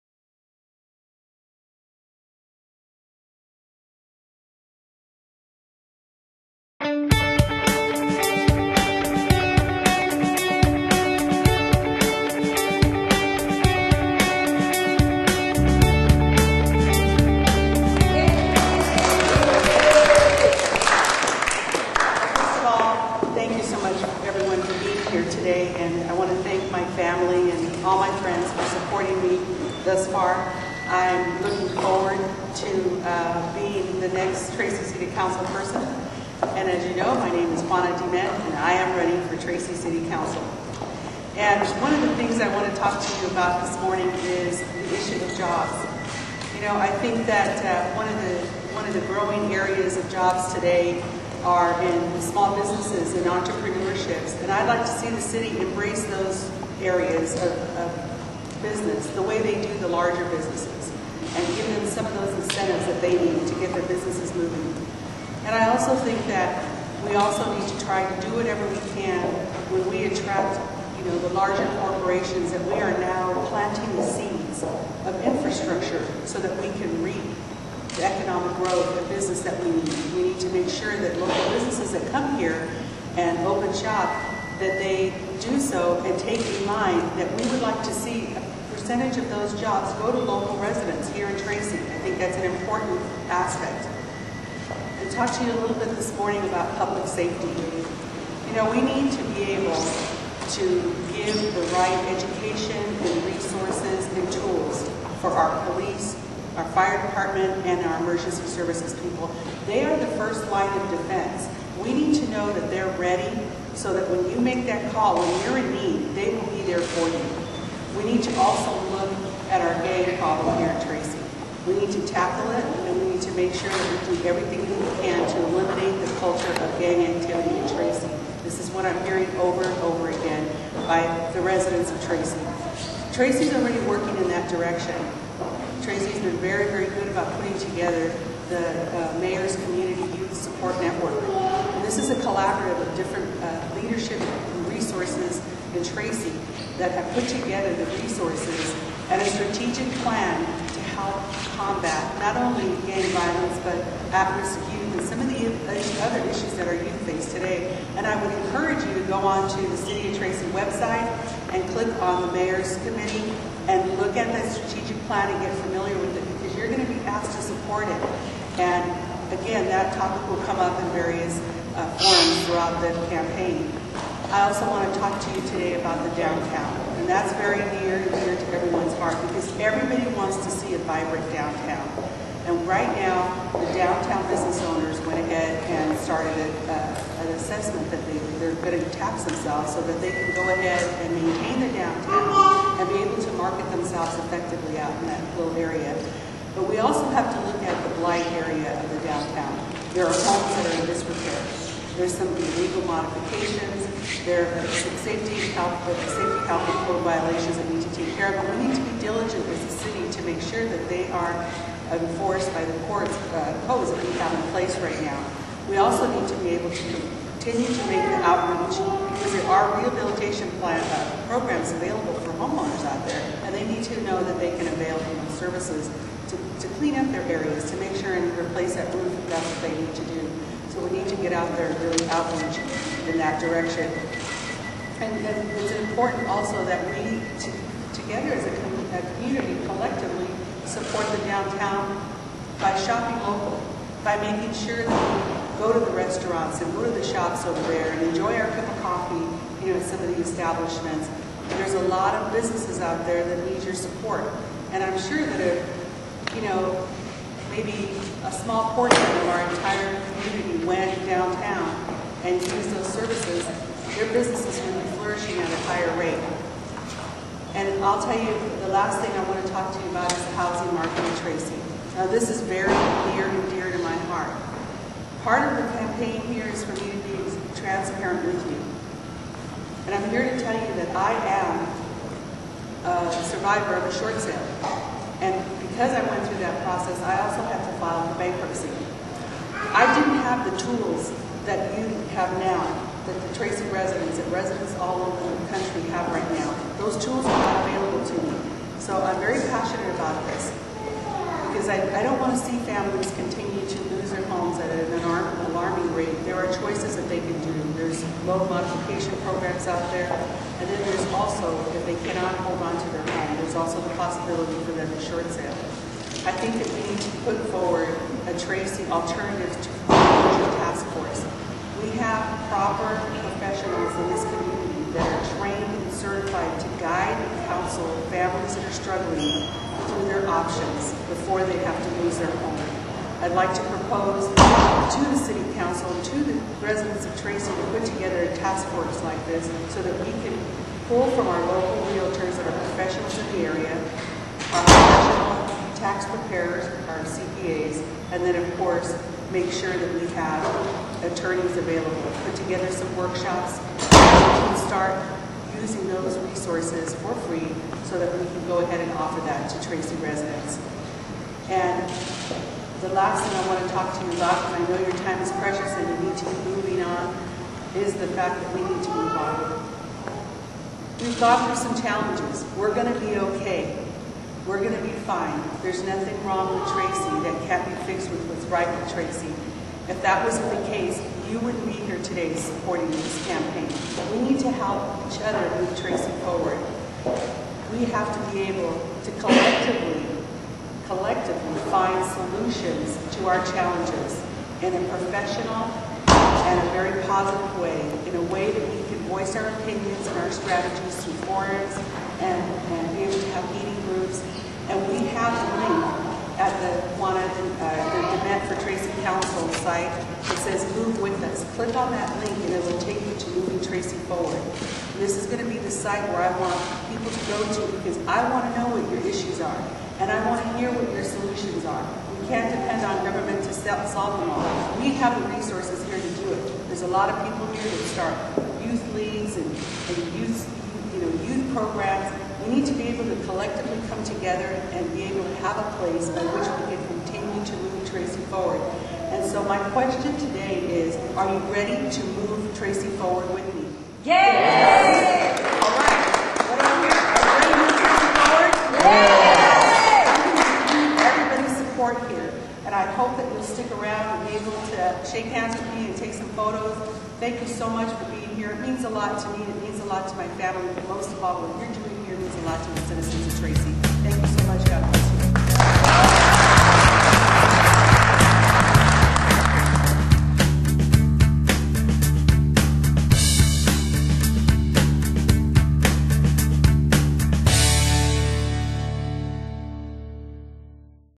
Juana Dement for City Council
Juana Dement announces her candidacy for City Council.